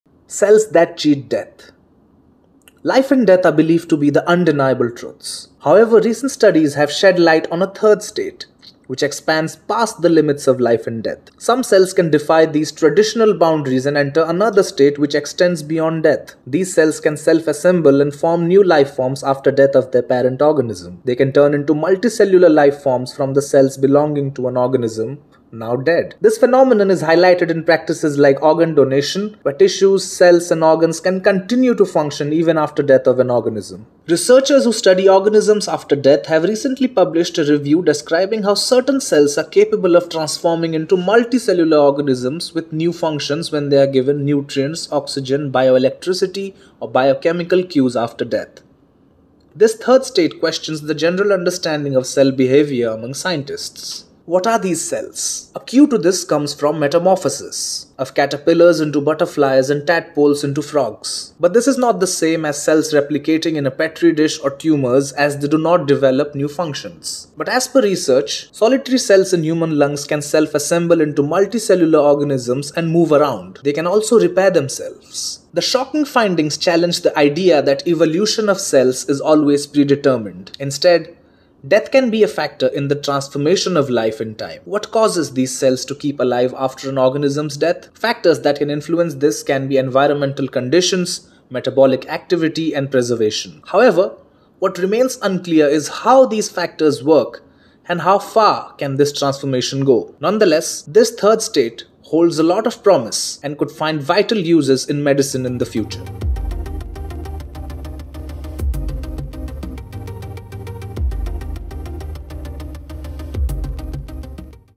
How Some Cells Can Survive Beyond Their Organism And Enter A 'Third State’ Between Life And Death
Science Explained: Ever wondered how some cells manage to endure beyond the life of their host and enter a mysterious 'third state' between life and death? Join us in this fascinating explainer video as we uncover the science behind these extraordinary cellular survival strategies!